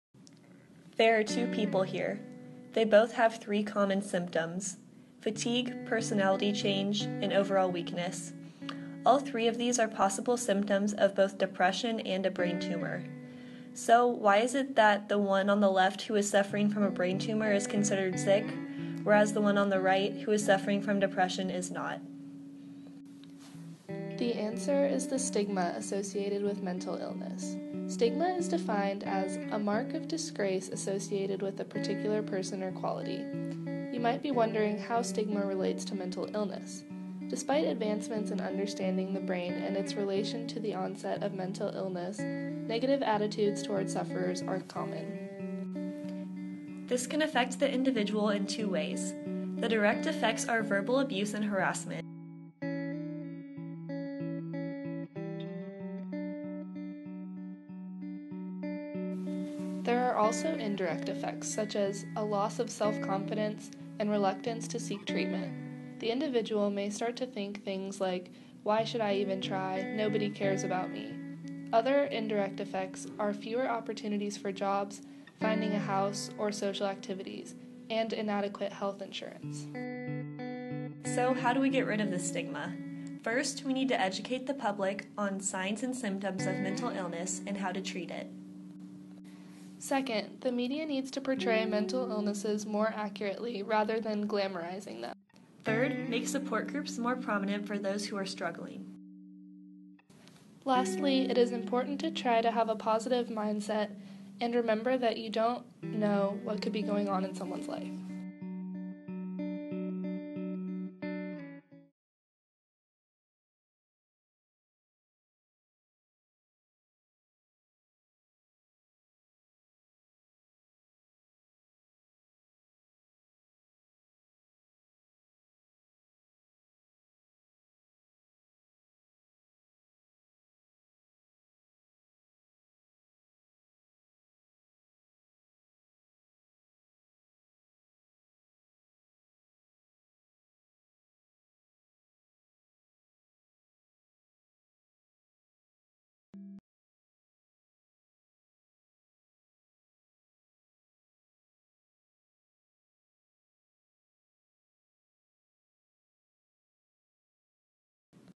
Stigma and Mental Illness
CAPS Neurology Project
Sources:
Corrigan, Patrick W., and Amy C. Watson. "Understanding the Impact of Stigma on People with Mental Illness." University of Chicago Center for Psychiatric Rehabilitation and Chicago Consortium for Stigma Research, n.d. Web. 13 Apr. 2017.
Mayo Clinic Staff. “Mental Health: Overcoming the Stigma of Mental Illness.” Mayo Clinic. N.p., 17 May 2014. Web. 13 Apr. 2017.
Corrigan, Patrick W. "On the Stigma of Mental Illness: Practical Strategies for Research and Social Change." American Psychological Association. American Psychological Association, n.d. Web. 13 Apr. 2017.